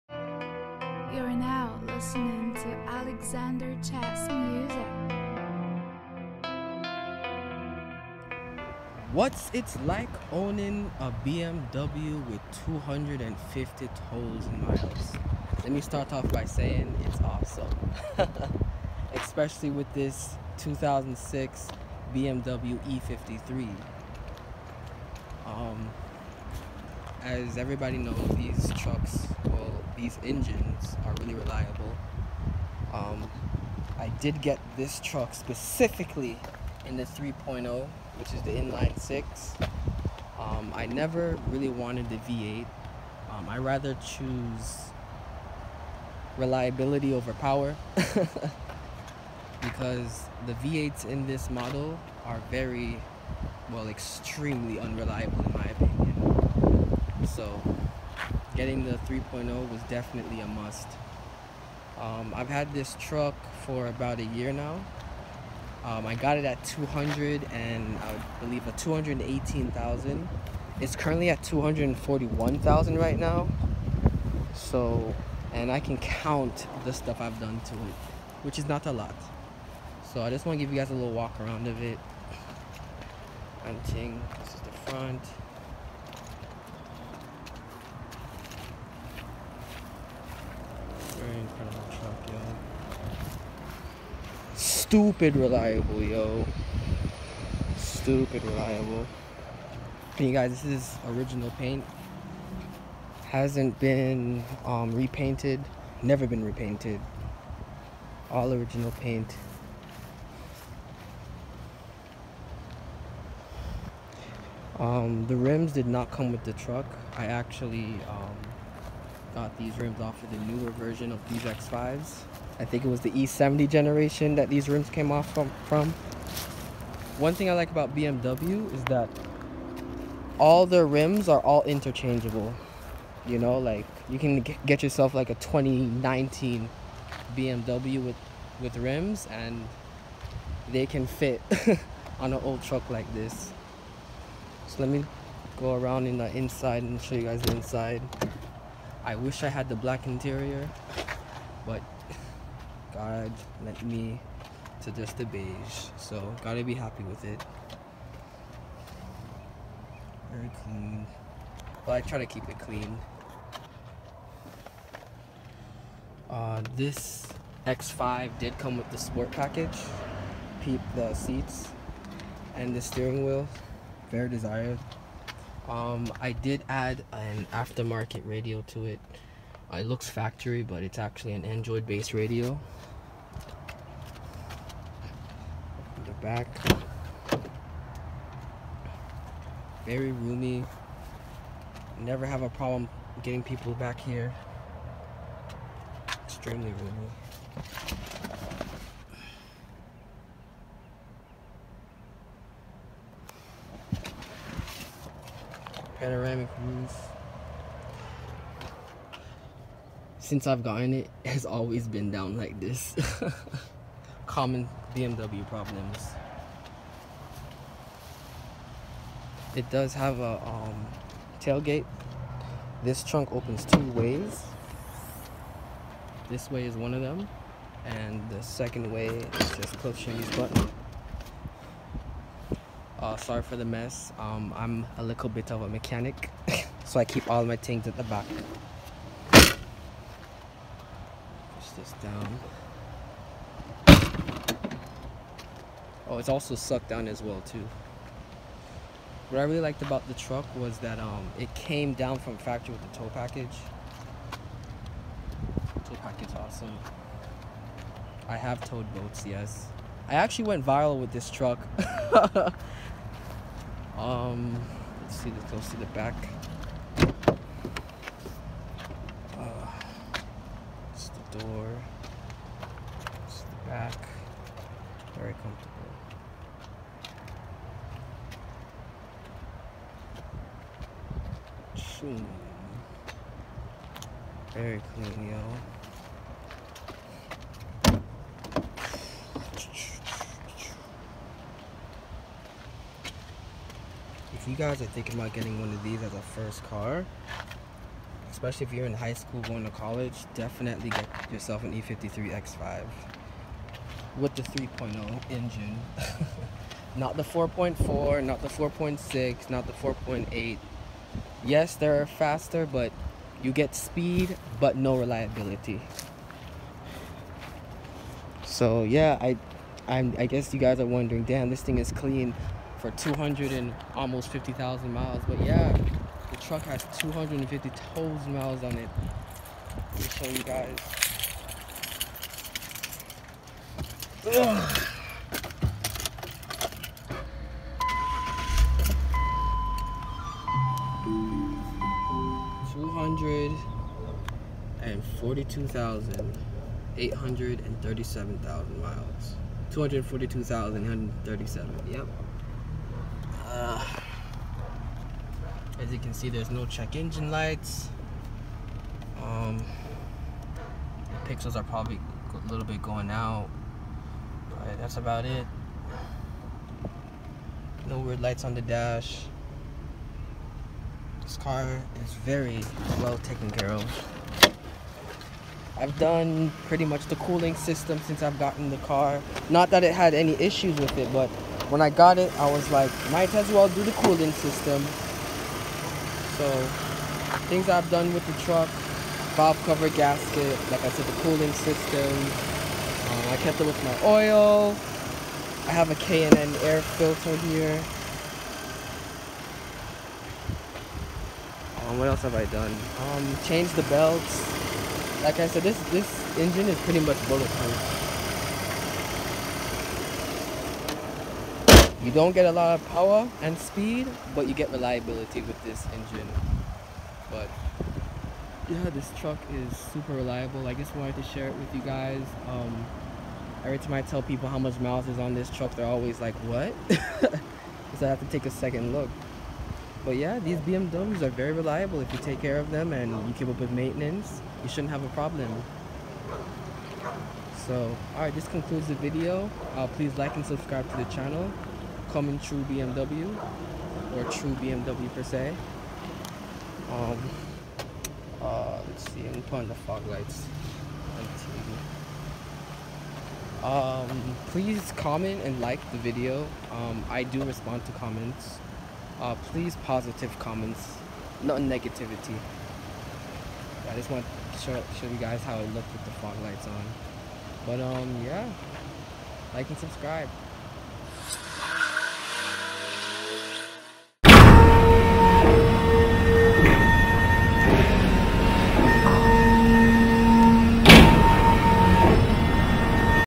Owning a BMW with 250,000 miles
This is my 2006 BMW E53 X5 3.0i. I’ll be doing a walk though of the car and giving full thoughts and recommendations.